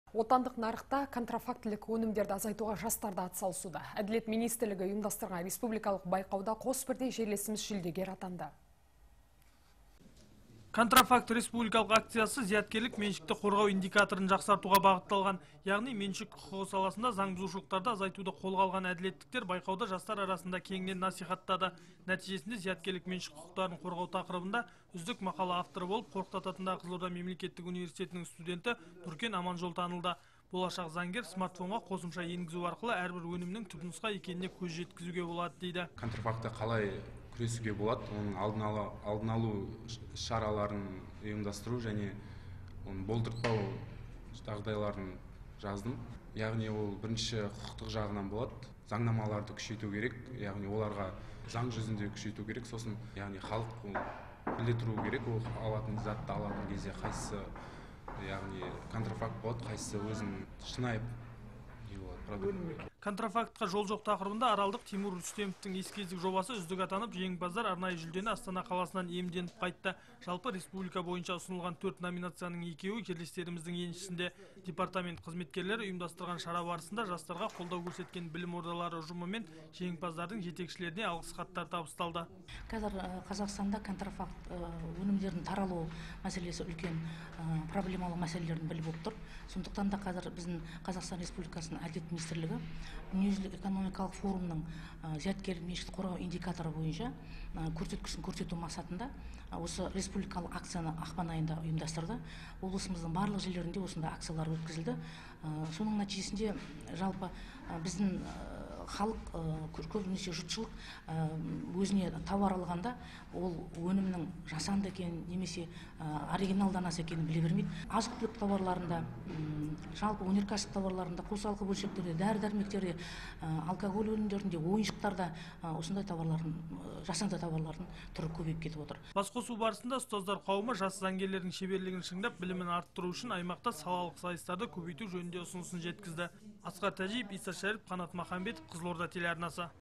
Жас заңгерлер үздіктер қатарында
Отандық нарықта контрафактілік өнімдерді азайтуға жастар да атсалысуда. Әділет министрлігі ұйымдастырған республикалық байқауда қос бірдей жерлесіміз жүлдегер атанды.